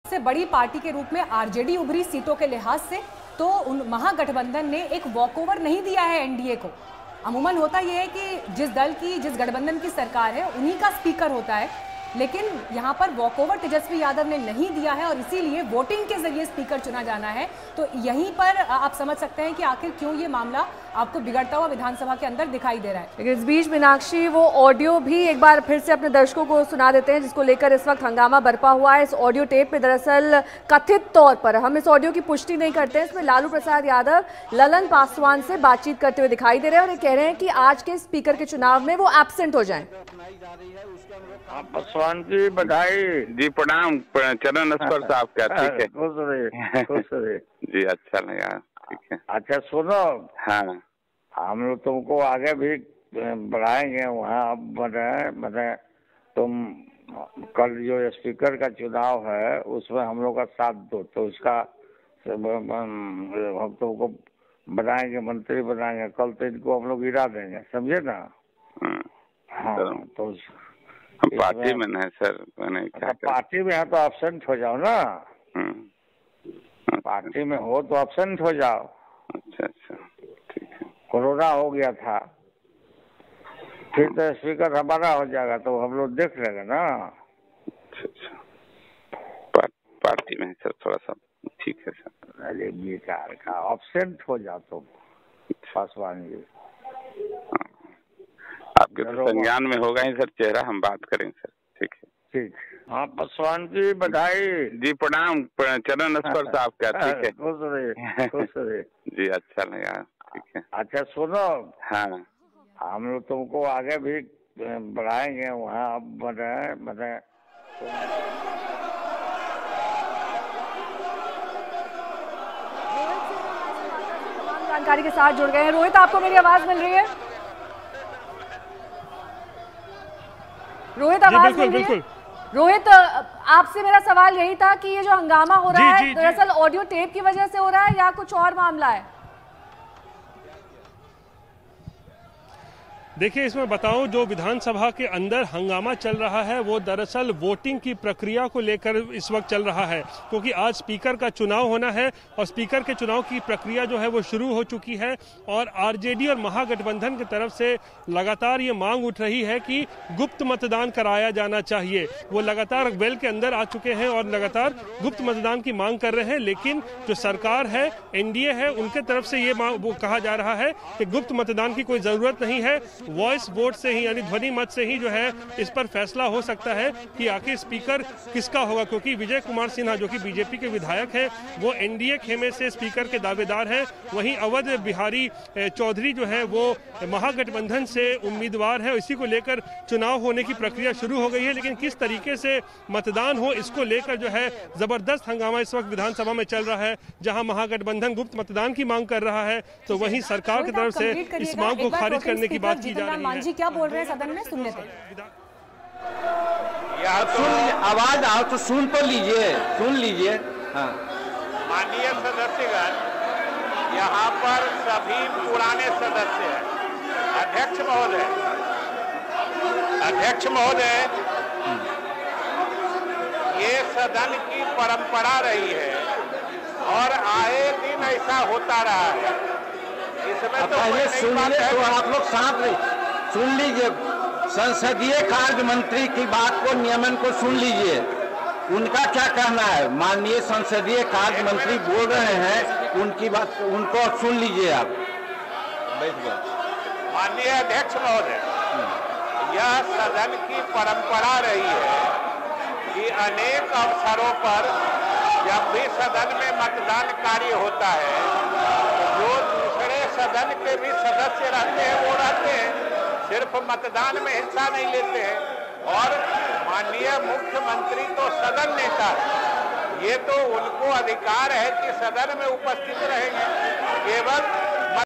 Bihar Speaker Election: विधानसभा में हंगामा, बीजेपी ने जारी किया लालू का कथित ऑडियो
बिहार विधानसभा में स्पीकर पद (Bihar Assembly Election Speaker) के चुनाव से पहले बीजेपी (BJP) और राजद (RJD) में आरोप-प्रत्यारोप का खेल जारी है. सुशील मोदी (Sushil Modi) ने पूर्व सीएम लालू यादव (Lalu Prasaad Yadav) पर बीजेपी नेताओं को जेल से फोन करने का आरोप लगाया है.